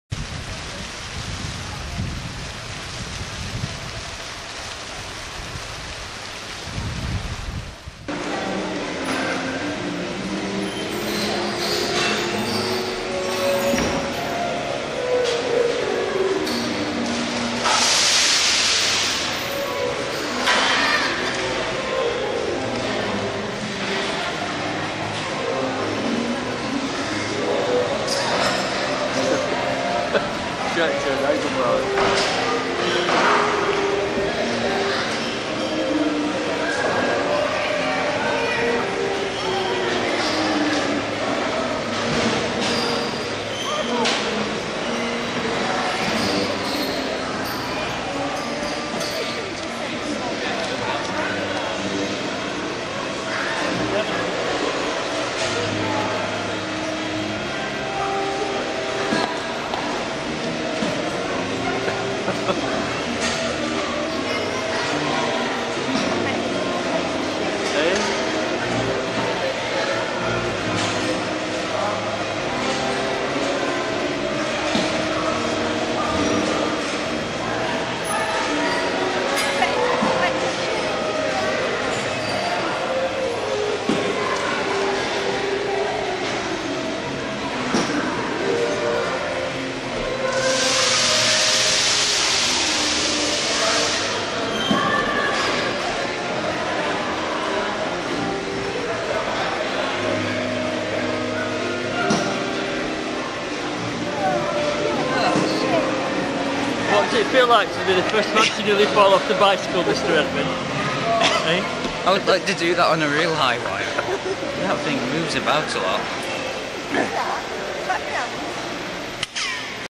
I do all my own stunts...
Seattle Science Center, 16 July 1991: I have a go on the "high wire" bicycle. In theory it shouldn't be possible to fall off, but I felt rather nervous when the counter weight started swinging.